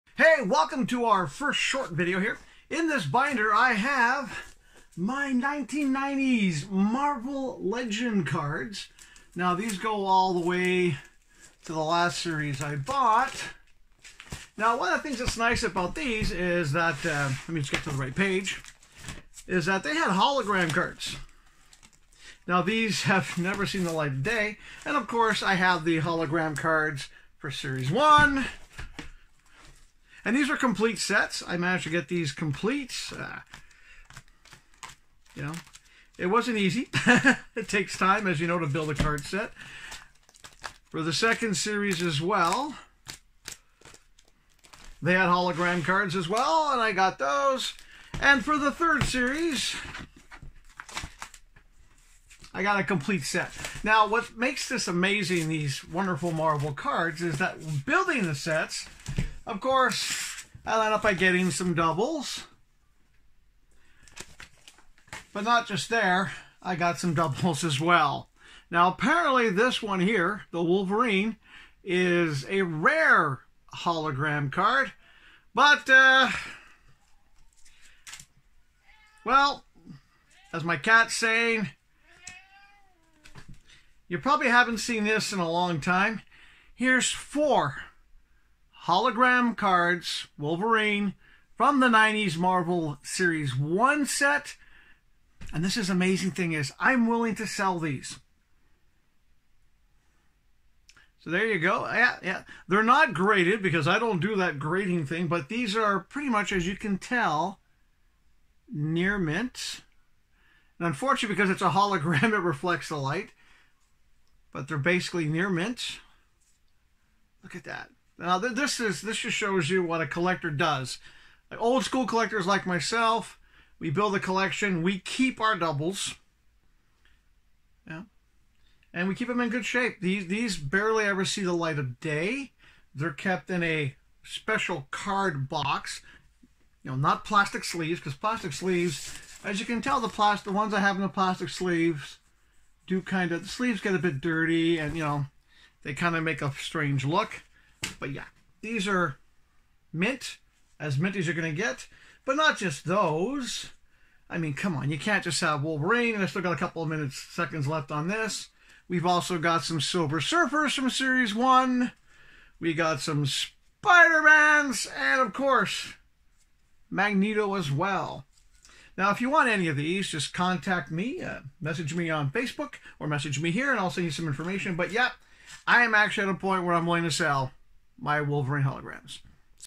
On The Peg Short #1
On The Peg Short #1 a quick look at my Marvel Trading cards including not 1 but 4 Wolverine Hologram cards,